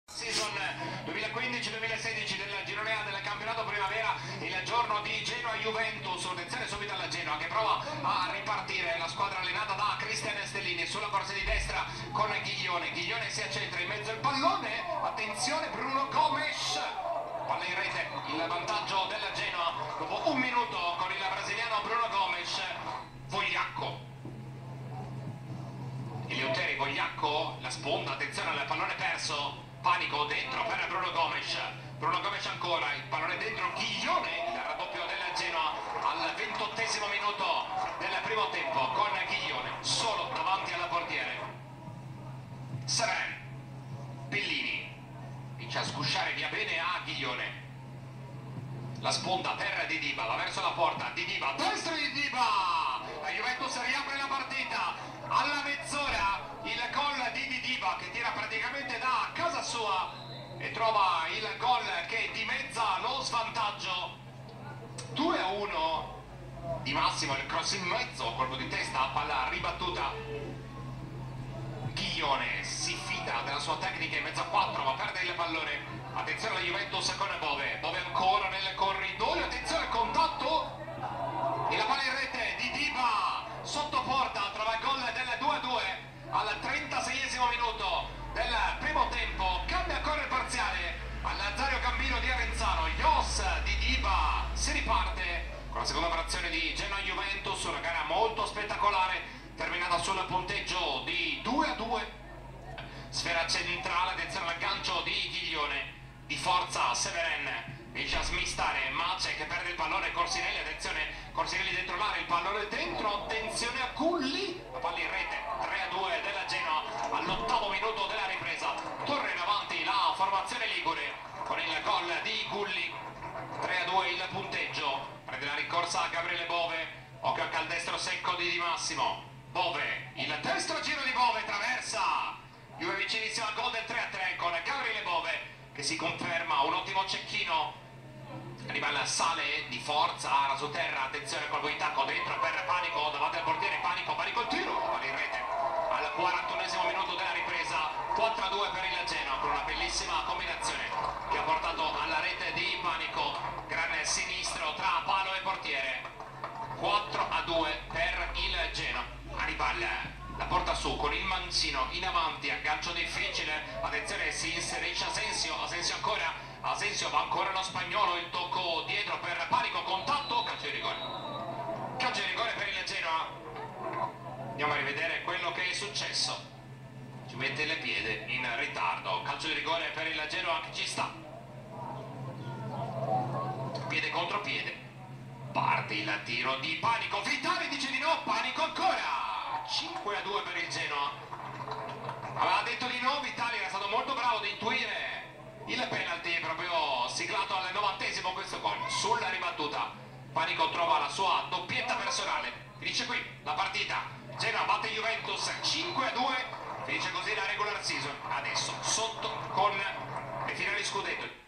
Primavera, Genoa-Juventus 5-2
Campionato Primavera, highlights Genoa-Juventus 5-2 (Gomes, Ghiglione, Gulli, Panico (2), Didiba (2)). Bianconeri chiudono il girone con una sconfitta. Cronaca e tabellino